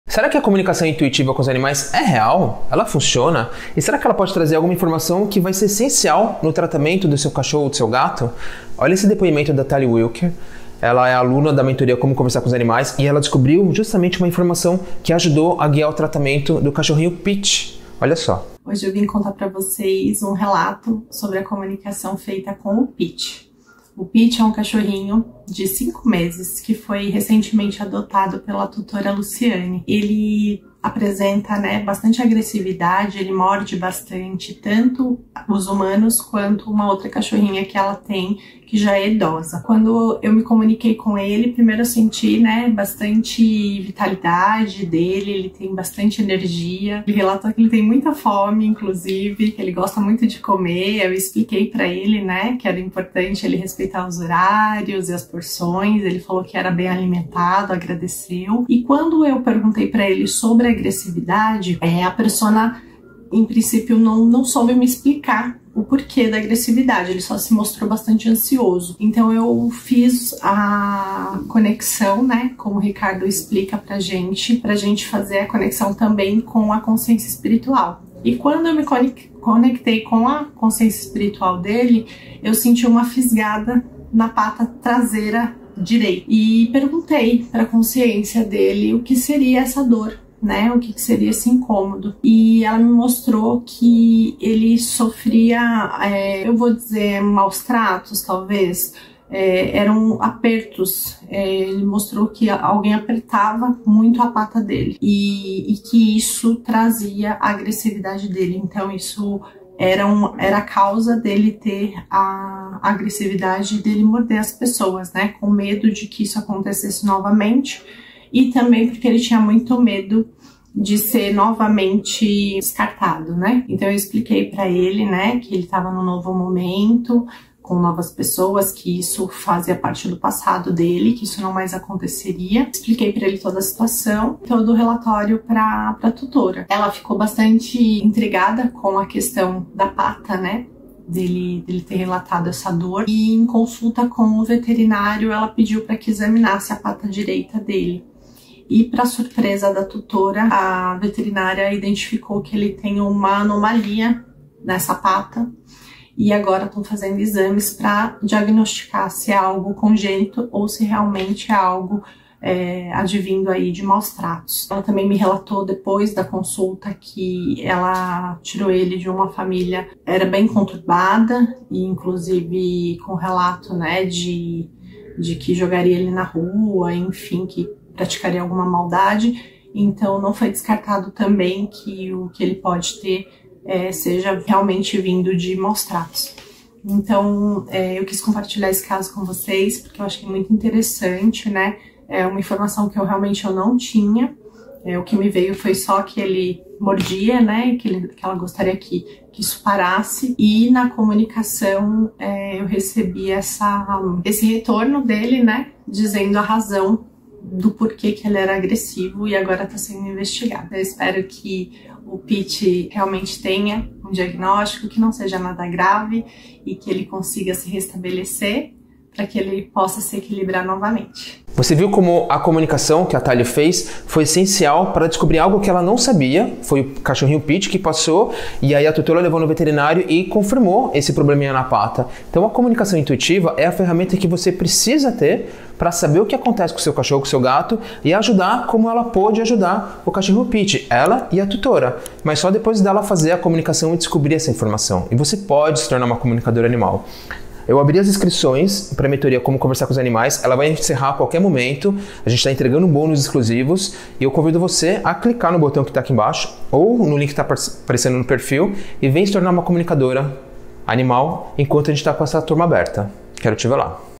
Será que a Comunicação Intuitiva com os animais é real?
Será que a Comunicação Intuitiva com os animais é real? Já teve aquela sensação de que você está falando com seu animal? Ou então quando olha para ele e sabe exatamente o que ele está falando? Isso é mais do que coincidência. Isso se chama comunicação intuitiva animal. E sim, ela é real e posso te ensinar o que é e como fazer.

Clique aqui para conhecer a Mentoria Como Conversar com os Animais com uma OFERTA ESPECIAL